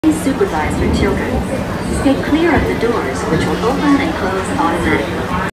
Lightnings caught on camera on the Disney People Mover.
I caught a lighting strike behind Space Mountain while riding the people Mover to avoid the storm.